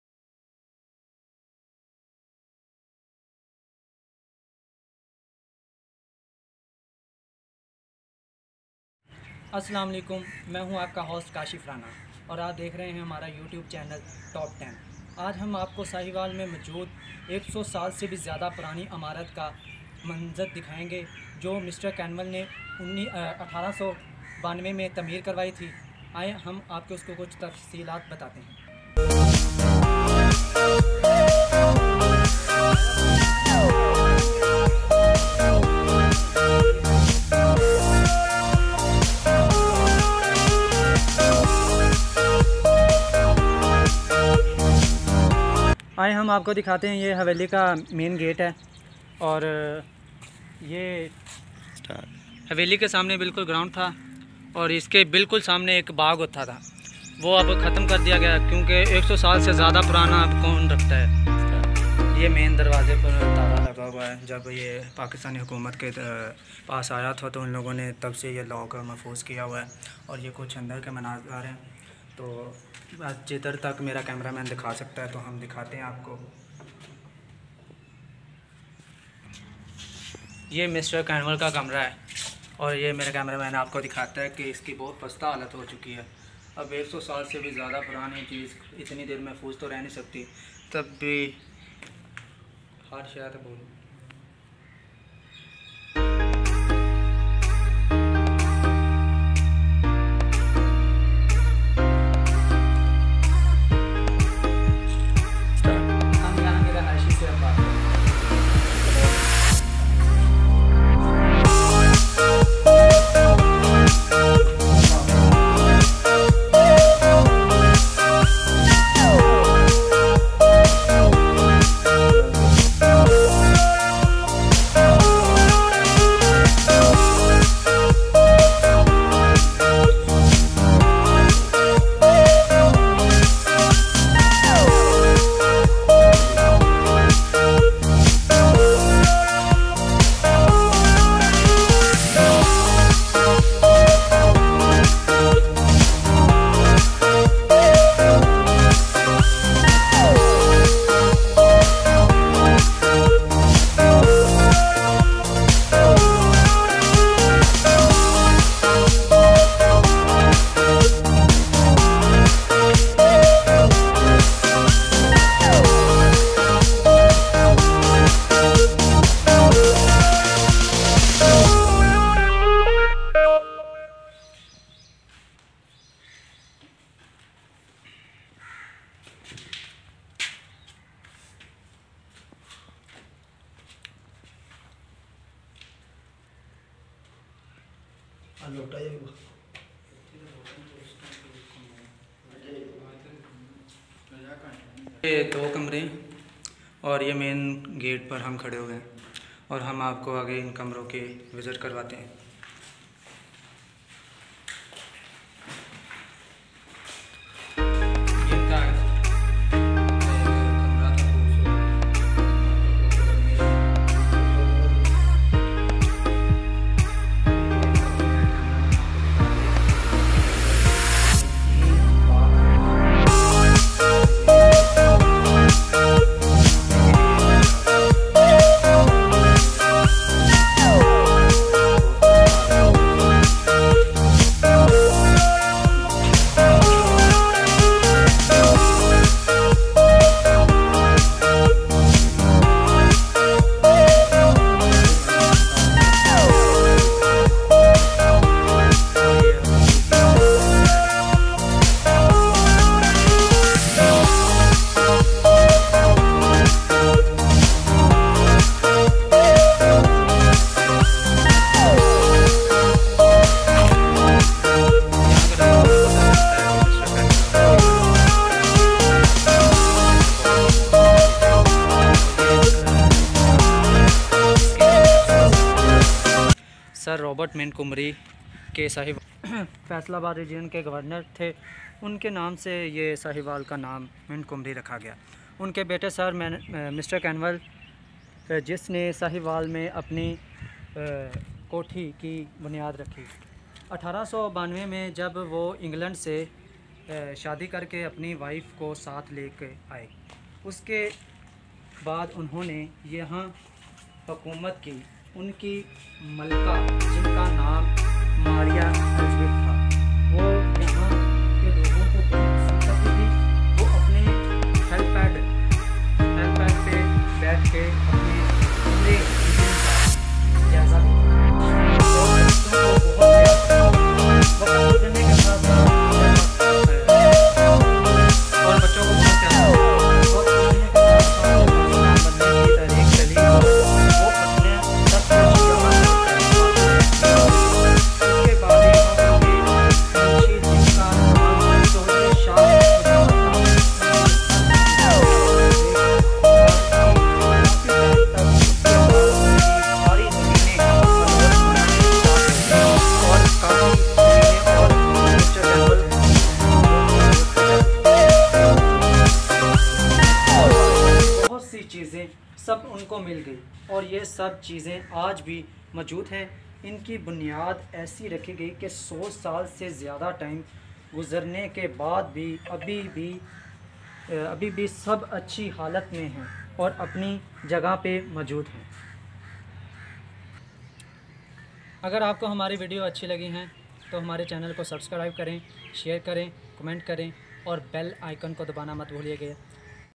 100 Saal Purani Angrez dour ki haveli | Historical place | MR Canval's Home
In this video i will share history of old home located at sahiwal.
old home build by angrez 100 years ago